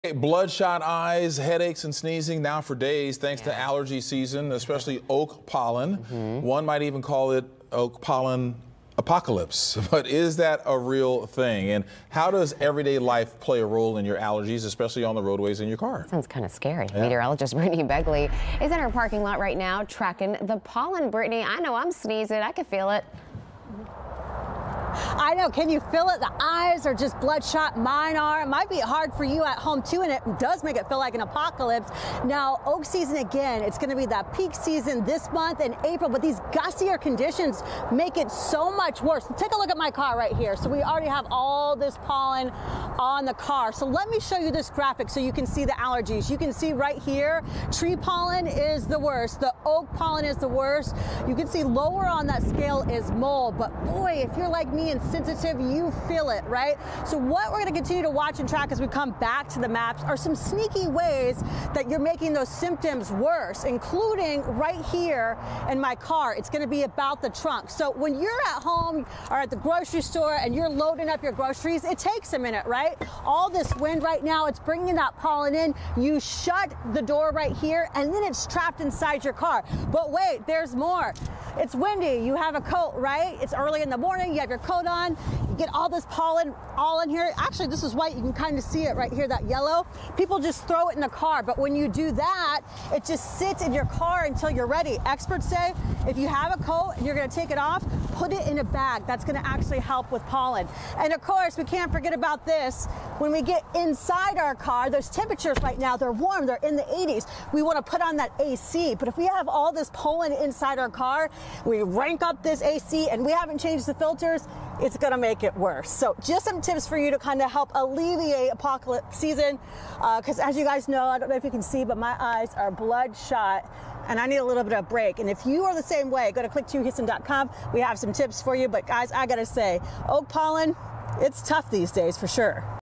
Oak Pollen Apocalypse: What You Need to Know
Allergy season hits hard with oak pollen—what’s really behind it?